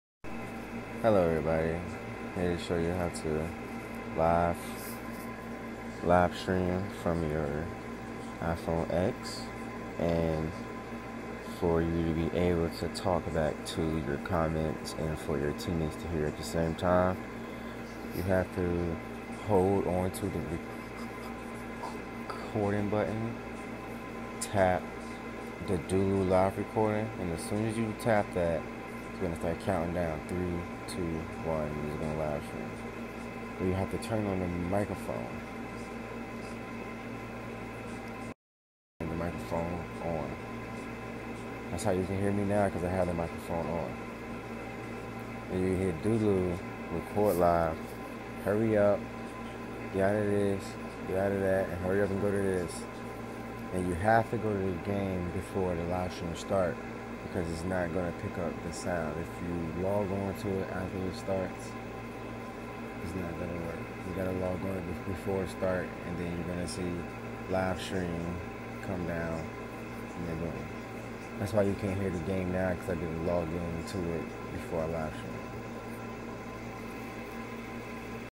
Du recorder live streaming tutorial how to make everyone hear you
How to use du recorder live. How to have your teammates and viewers hear you tutorial for the du recorder live app .- I HAVE A IPHONE X, IT MAY BE DIFFERENT FROM ANOTHER DEVICES